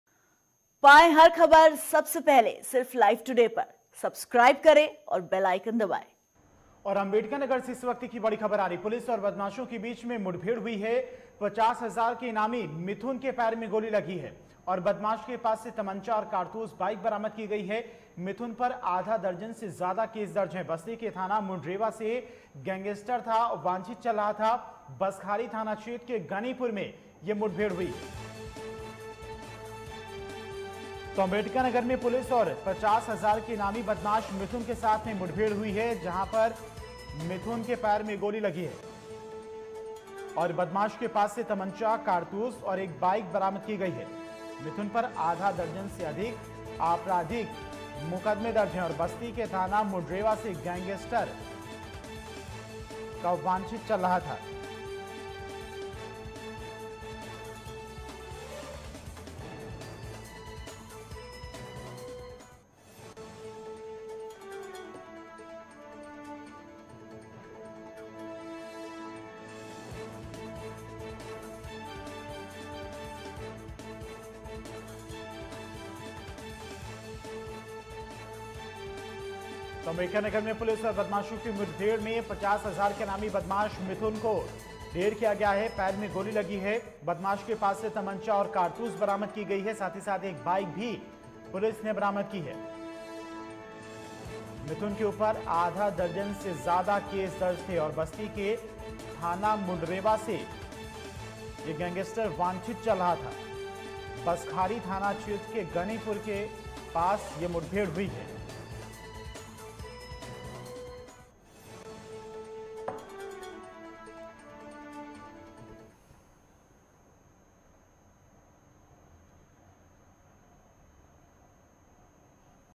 Ambedkar Nagar में पुलिस और बदमाशों के बीच मुठभेड़, 50 हज़ार के इलामी मिथुन हुआ घायल
हमसे जुड़े रहने के लिए हमें फॉलो करें